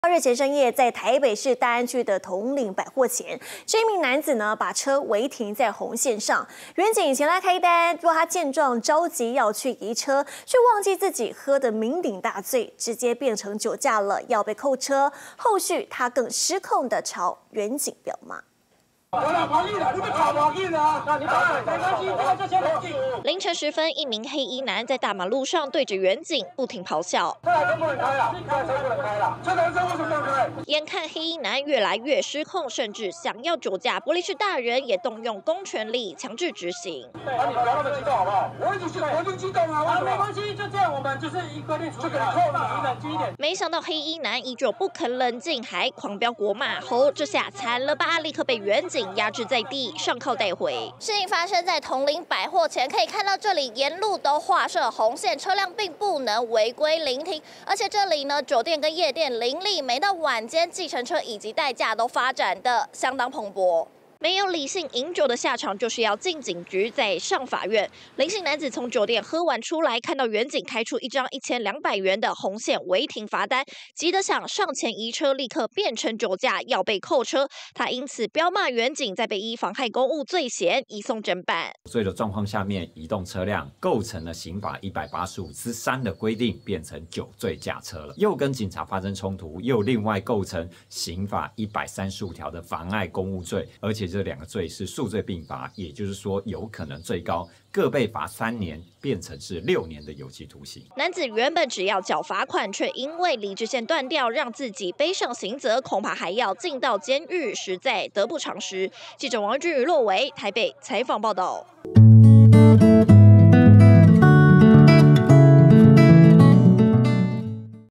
獨家／紅線違停移車變酒駕！ 醉男暴氣辱警遭壓制
日前深夜在台北市大安區統領百貨前，有名男子把車違停在紅線上，員警前來開單，男子見狀急得要去移車，卻忘了自己喝的酩酊大醉，直接變成酒駕，要被扣車，後續他更失控的朝員警飆罵，再被依妨害公務罪嫌移送偵辦。